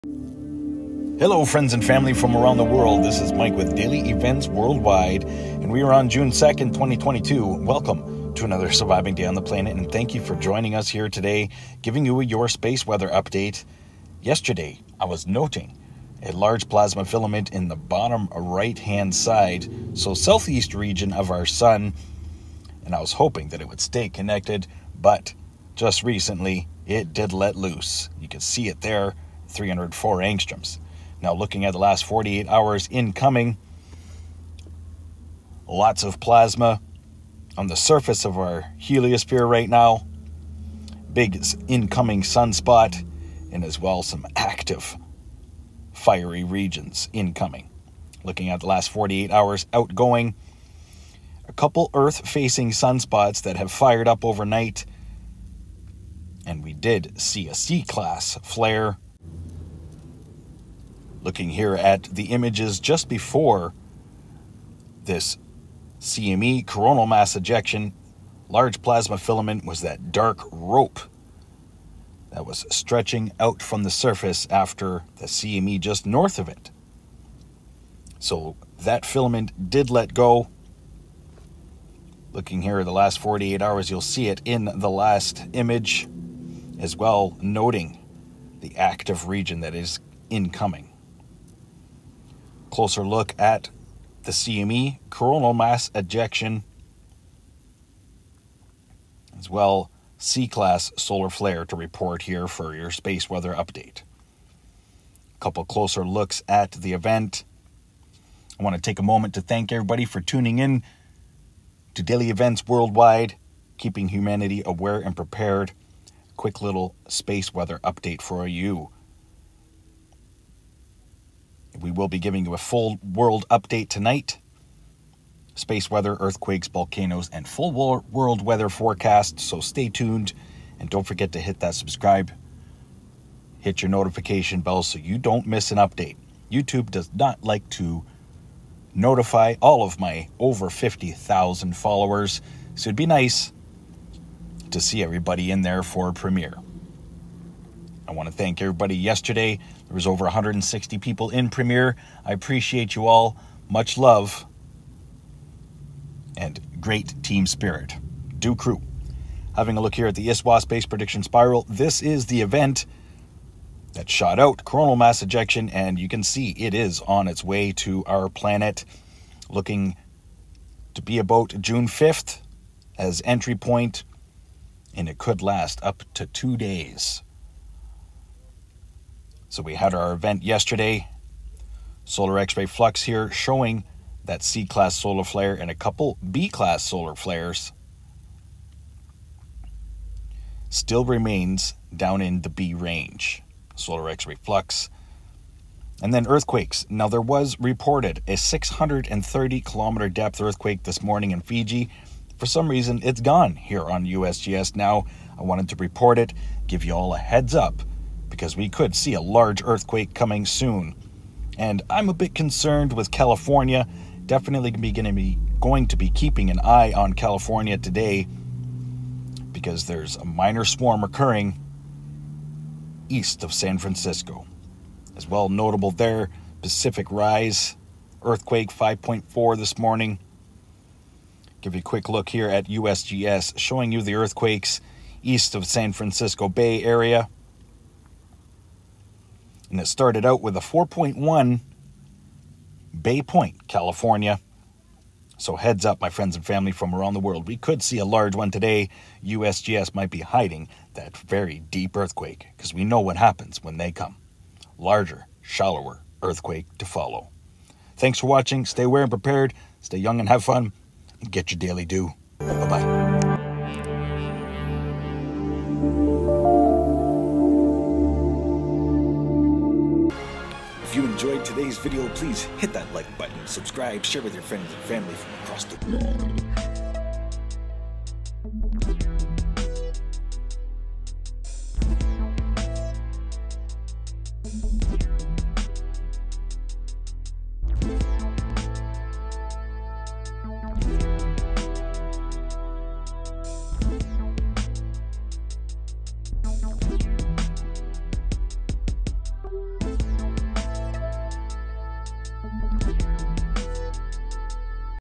LARGE C.M.E. Earth Directed! / Earthquake WATCH In Effect!
June 2 2022

A large CME blast Earth directed was detected departing from the SE region of Our Sun Today.
Earthquake WATCH in effect!  DEEP Earthquake reported Fiji, than quickly taken down by USGS.